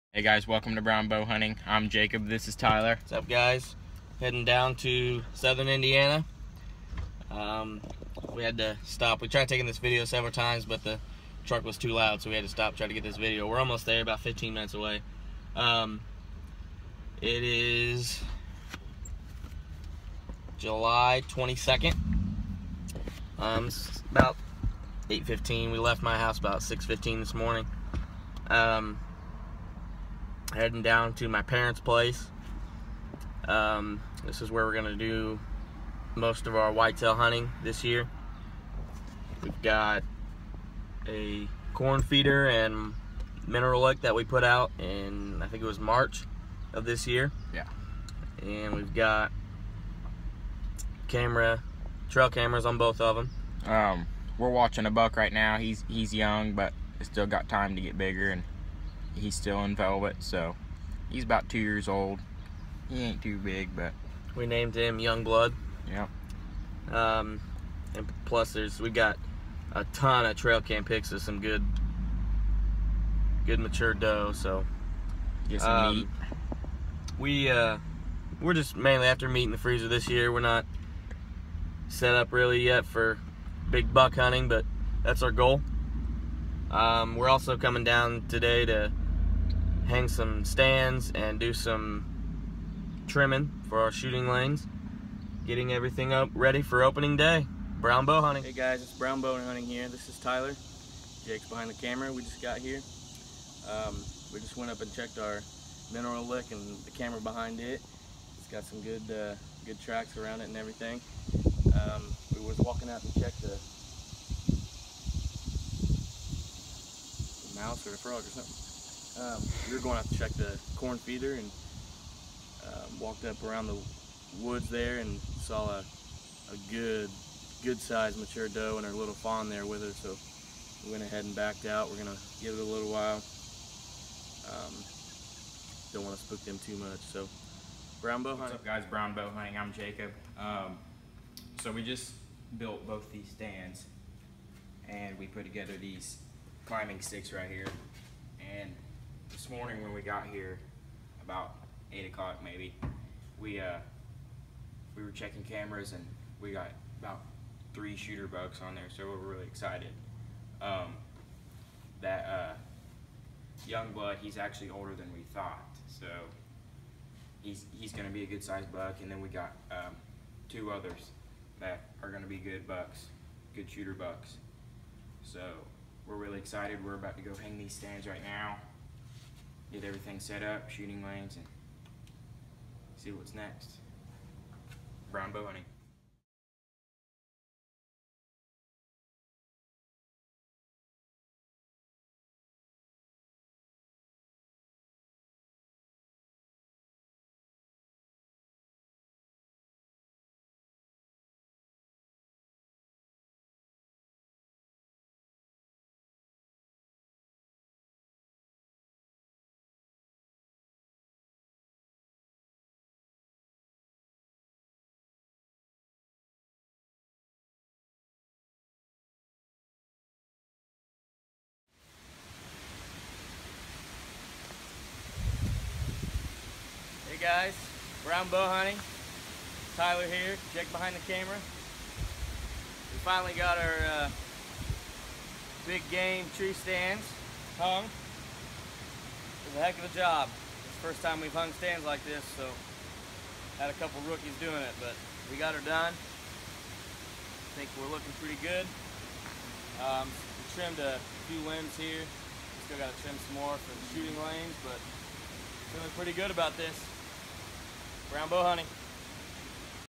Hanging Stands, Trimming Lanes and Checking Cameras
Brown Bowhunting preparing for opening day of bow season. Hanging tree stands, trimming shooting lanes, and checking trail cameras.